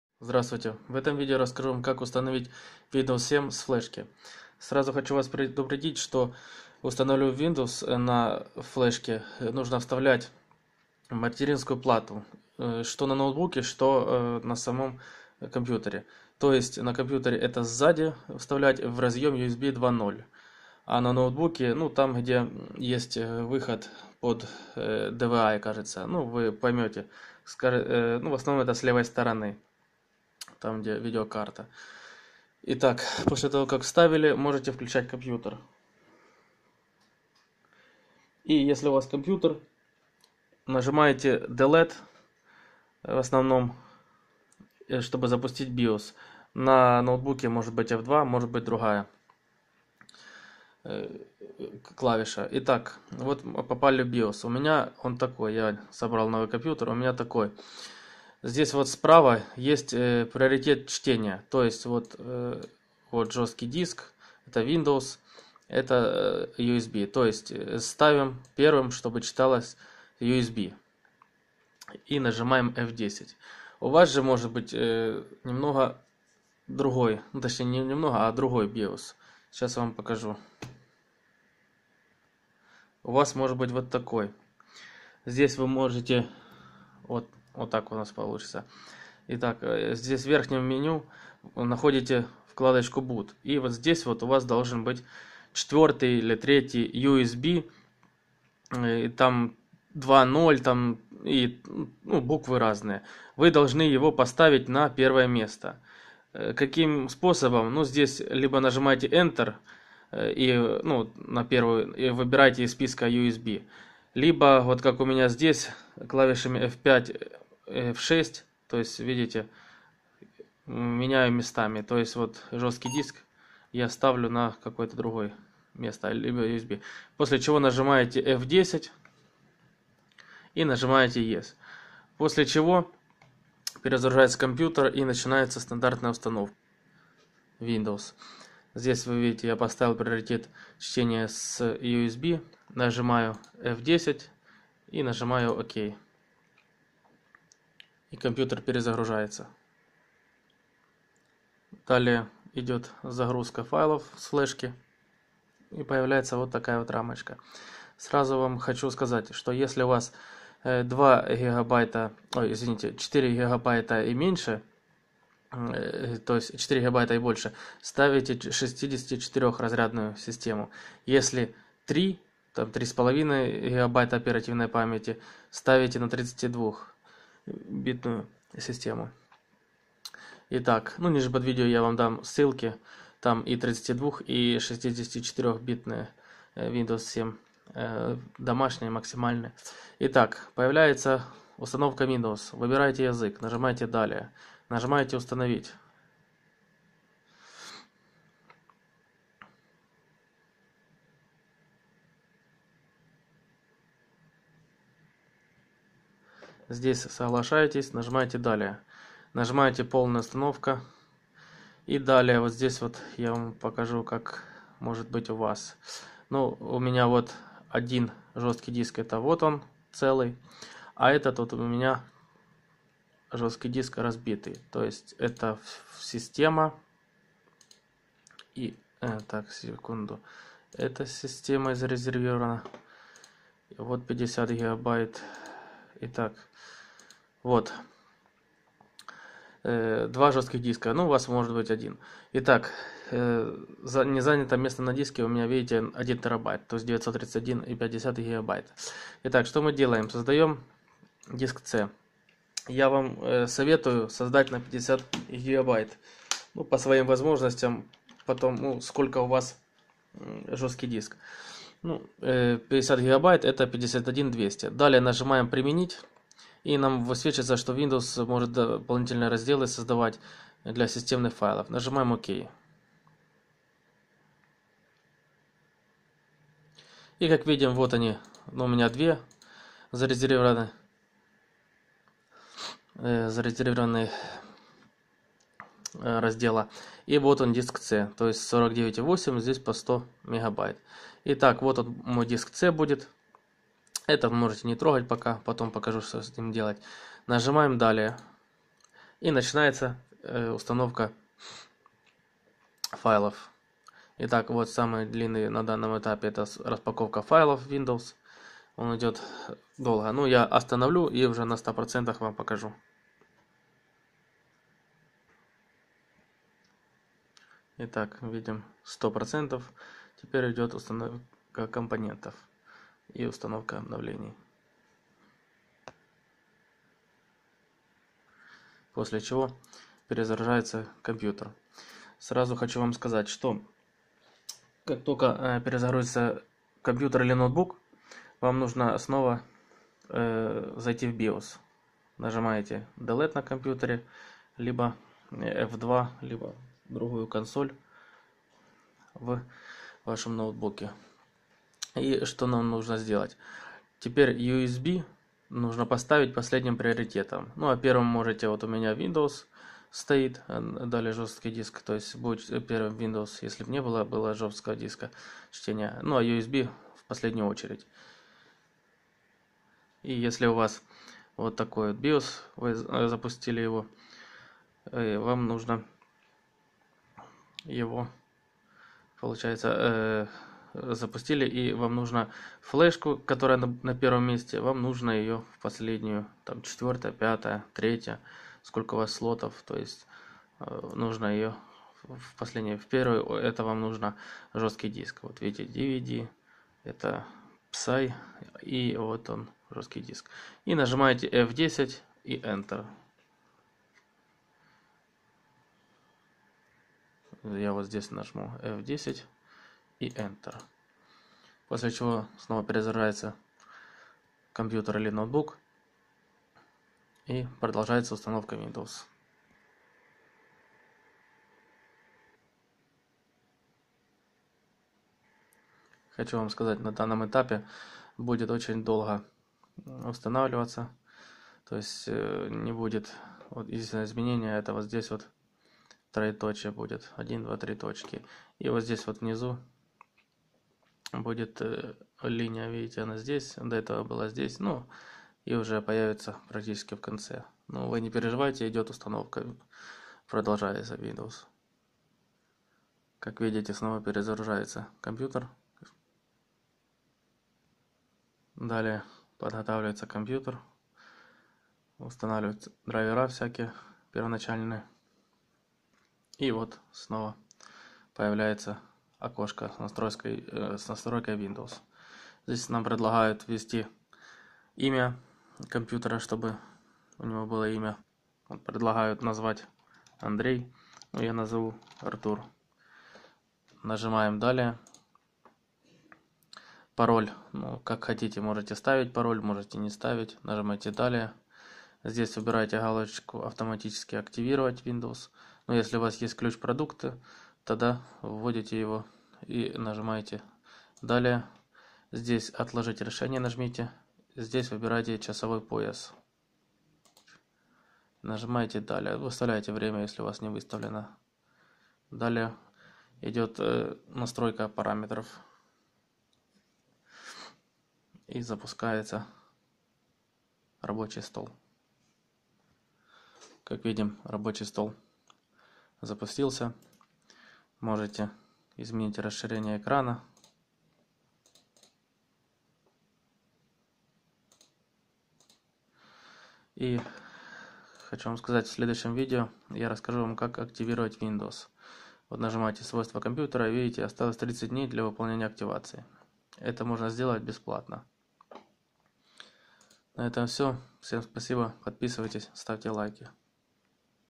Установка Windows 7 Home (Домашняя)64 бита на компьютер)
Ссылка на программу для записи WinToFlesh

Windows 7 32 бита (86) Домашняя (с гугл диска)

Windows 7 64 бита (64) Домашняя (с гугл диска)